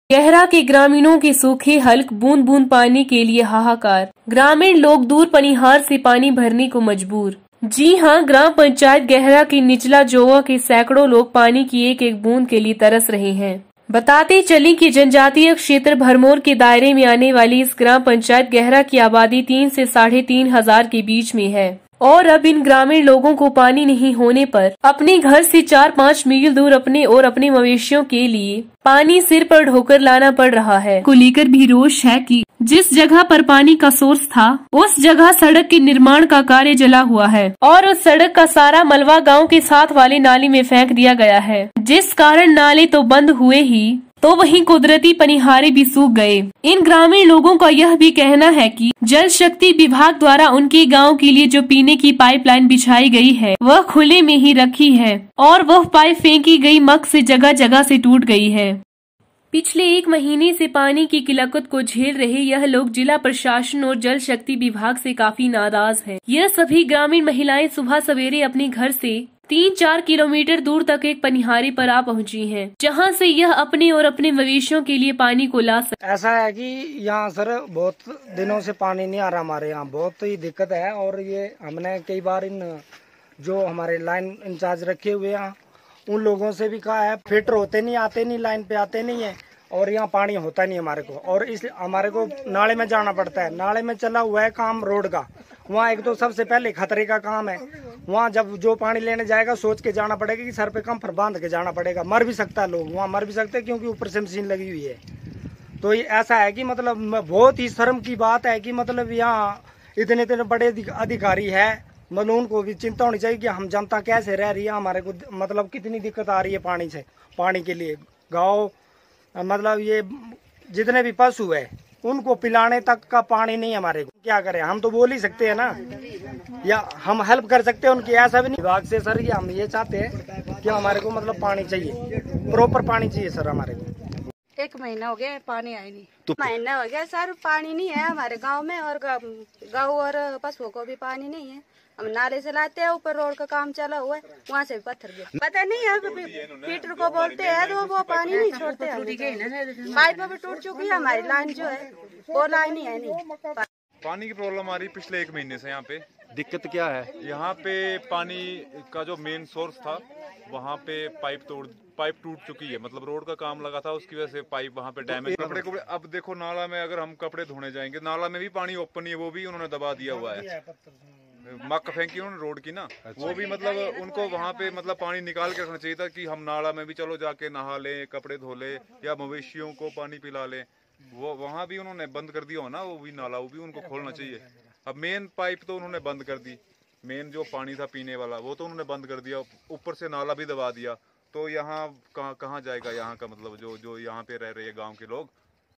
सड़क निर्माण के चलते पाइप लाइन क्षतिग्रस्त पानी की बूंद बूंद को तरस रहे लोग Tribal Today Tv
विधानसभा क्षेत्र भरमौर के अंतर्गत आने वाली ग्राम पंचायत गैहरा के निचला जौवा गांव के लोग पानी की बूंद बूंद को तरस रहे है लोगों का कहना है कि सड़क निर्माण के चलते उनके गांवो को आने वाली पाइप लाइन टूट चुकी है जिसकी वजह से करीब तीन हजार की आबादी मीलों दूर से पानी ढोने को मजबूर है ग्रामीणों के अनुसार पीने का पानी तो दूर उन्हे अपने मवेशियों की प्यास बुझाने के लिए भी मीलों दूर से पीठ पर पानी ढोना पड़ रहा है पिछले एक माह से पानी की किल्लत झेल रहे लोग जिला प्रशासन और जल शक्ति विभाग से भी खासे नाराज हैं ग्रामीणों ने प्रशासन और जल शक्ति विभाग से मांग की है कि निचला जौवा गांव को जल्द से जल्द पानी की आपूर्ति सुचारू रूप से बहाल की जाए।
peoplearecravingwaterfromone month